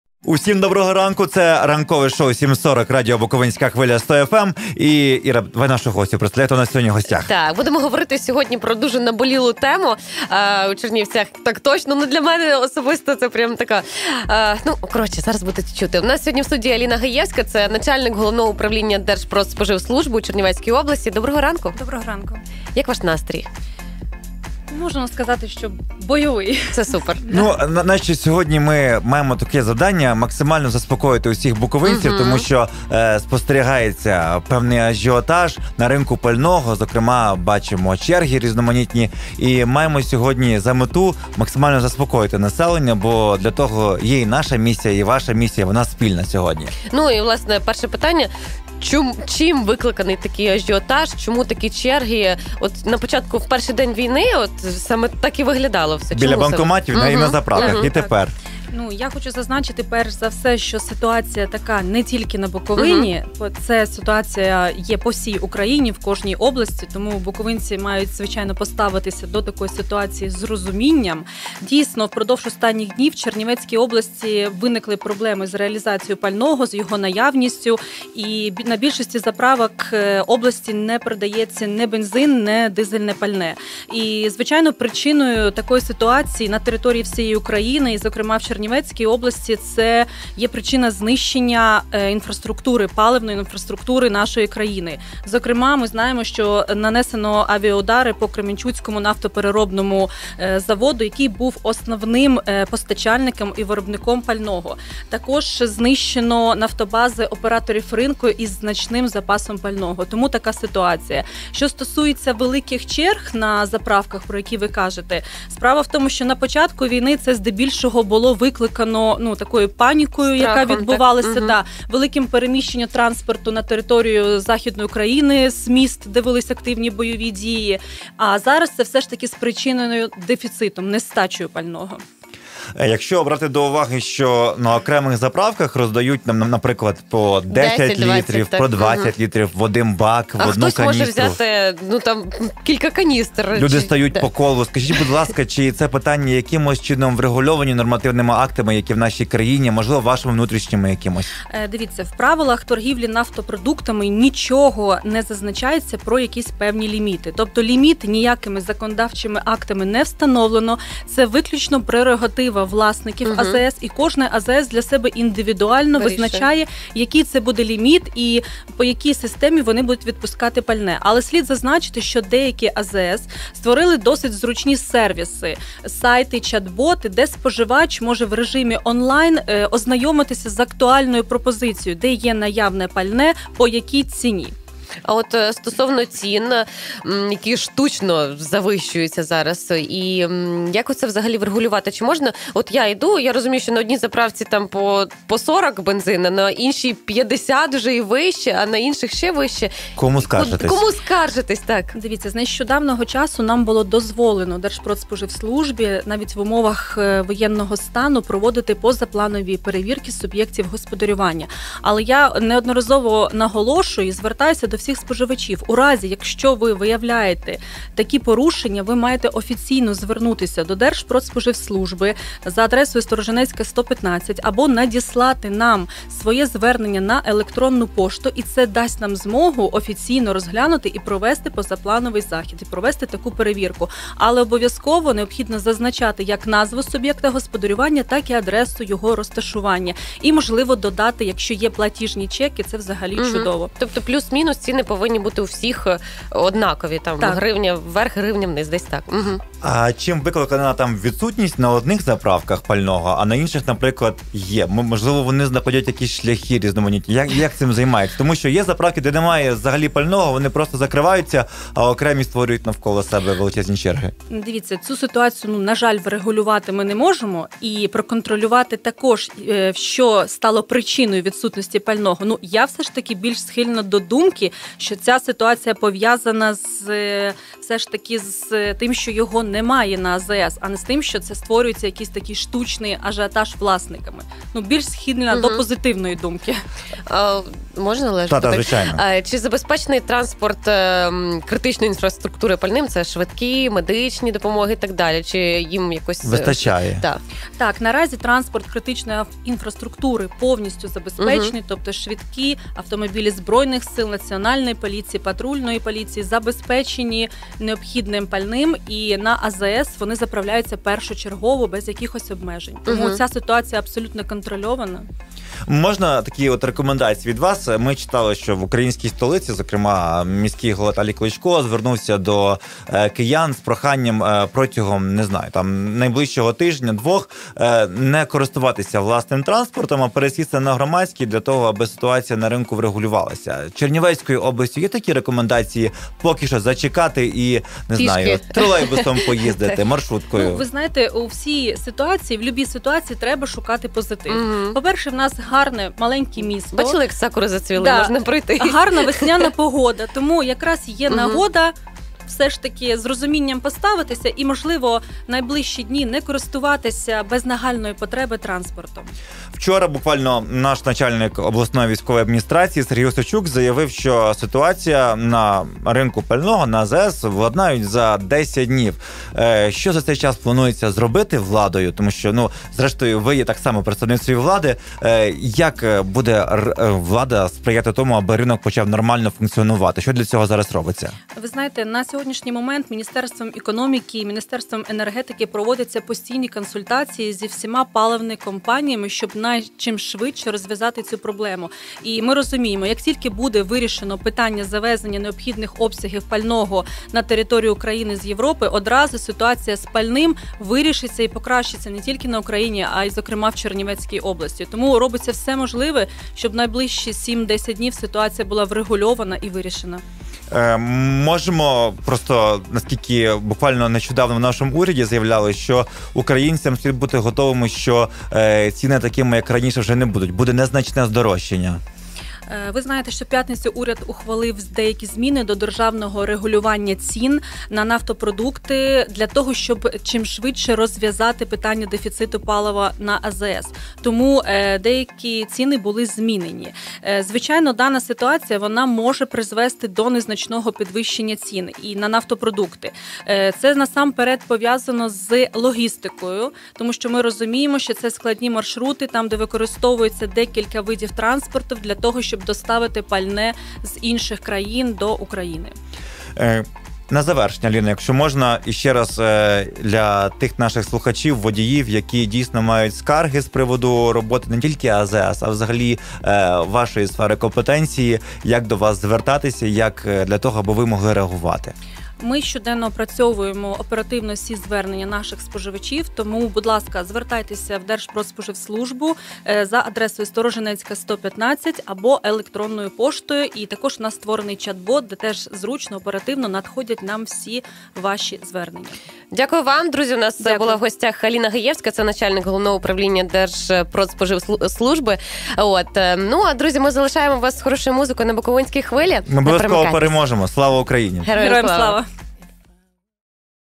Аліна Гаєвська про стан ринку пального на Буковині
Сьогодні вранці спілкувалися про ринок пального на Буковині. На запитання слухачів відповідала начальник Головного управління Держпродспоживслужби у Чернівецькій області – Аліна Гаєвська. Посадовиця розповіла про конкретні кроки влади, які спрямовані на стабілізацією ситуації.